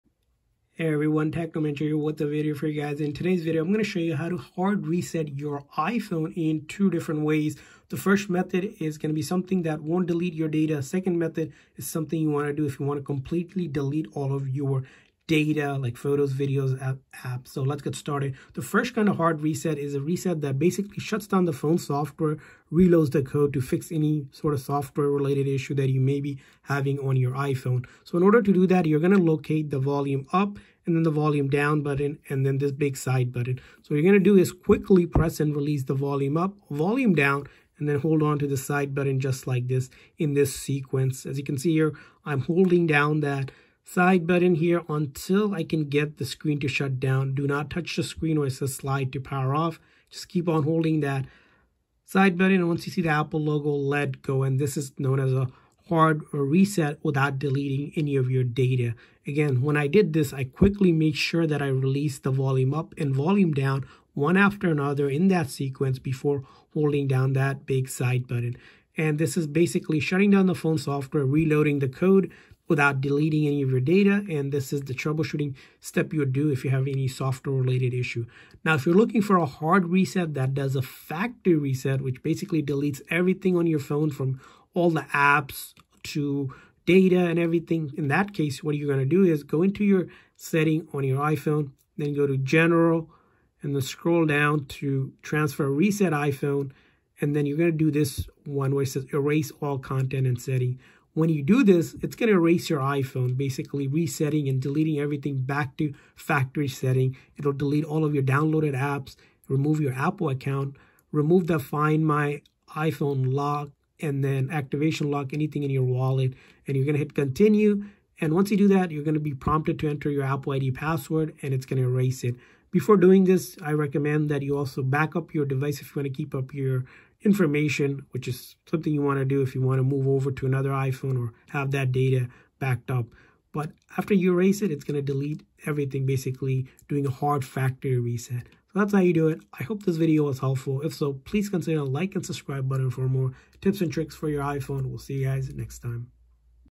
How To Hard Reset iPhone?
In this tutorial, you will learn how to hard reset an iPhone!